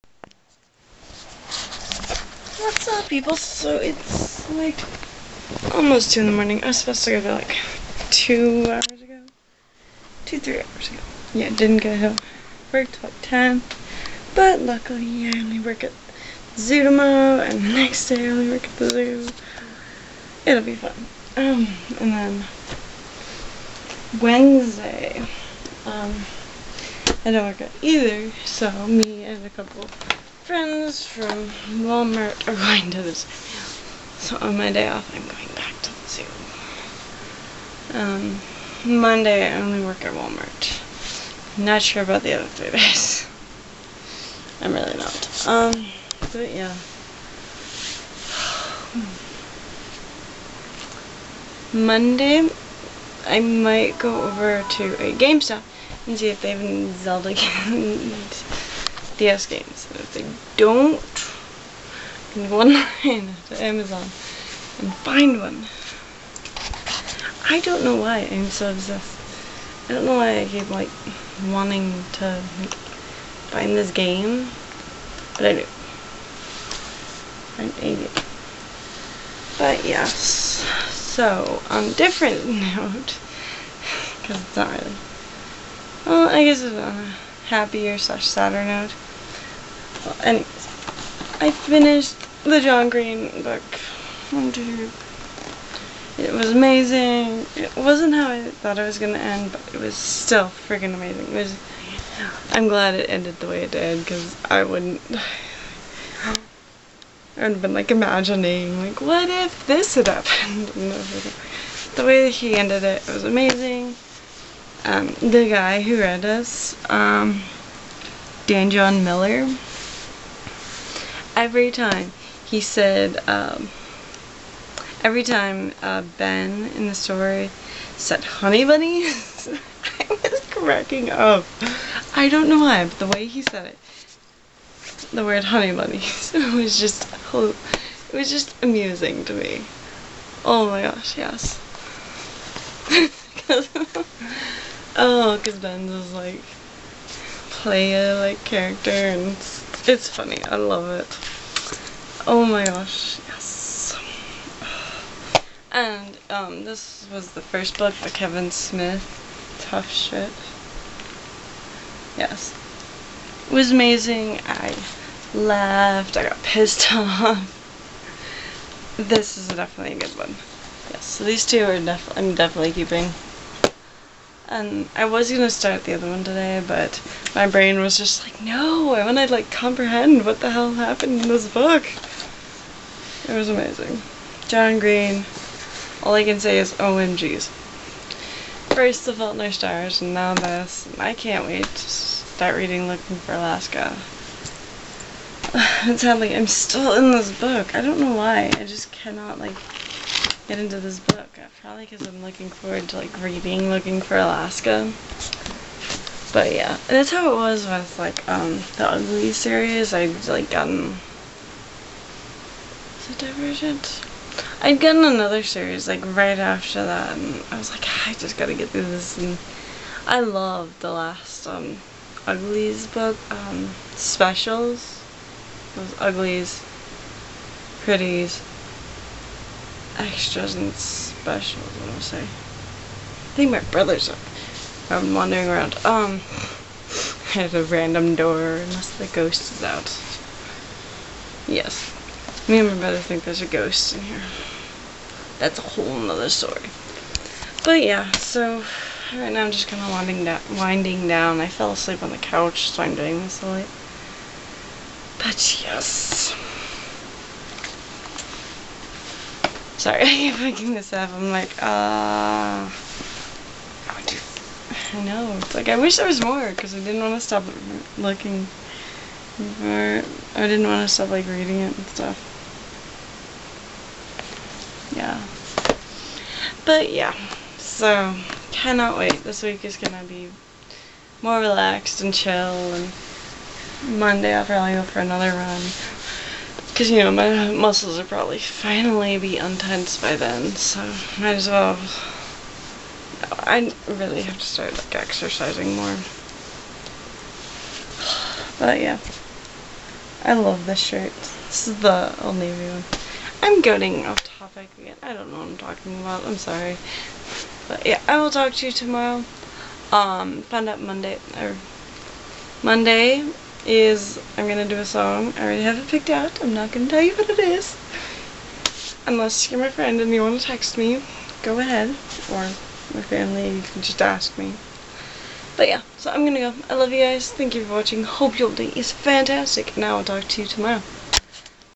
Paper towns (Vlog 147)
Hello! So I finished listening to the audio version of John Green's "Paper Town" and I can't wait to read the book for myself! John Green's writing style is amazing! Hope your day is great!